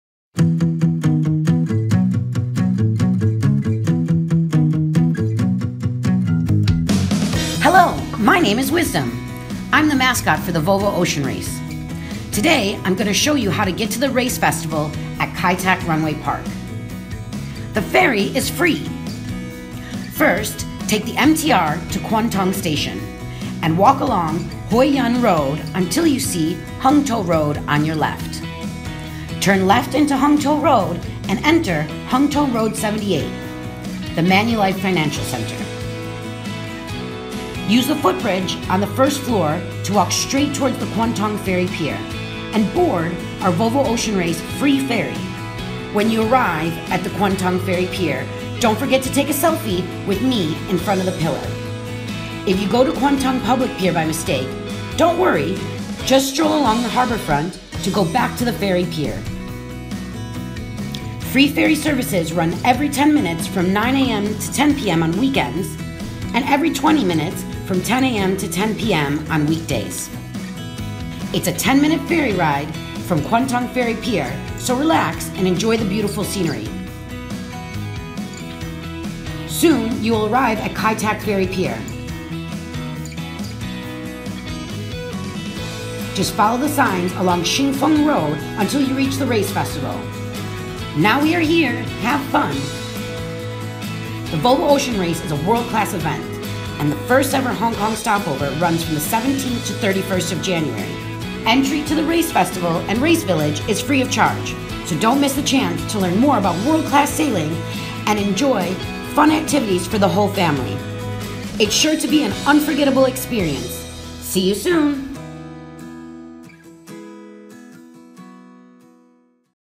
WISDOM SHOWS THE WAY TO VOLVO OCEAN RACE (2018)
Volvo Ocean Race Hong Kong Stopover opens tomorrow! Wisdom, the event’s mascot, shows us how to take a free ferry from Kwun Tong Ferry Pier to the Race Village, where a Race Festival is packed with fun activities for all the family. Watch the video and let Wisdom lead the way.

The Race Festival is open to the public for free from tomorrow until 31 January. Free ferry services at Kwun Tong Ferry Pier run every 10 minutes from 9 a.m. to 10 p.m. on weekends, and every 20 minutes from 10 a.m. to 10 p.m. on weekdays. It’s a 10-minute ferry ride to Kai Tak Runway Park Pier.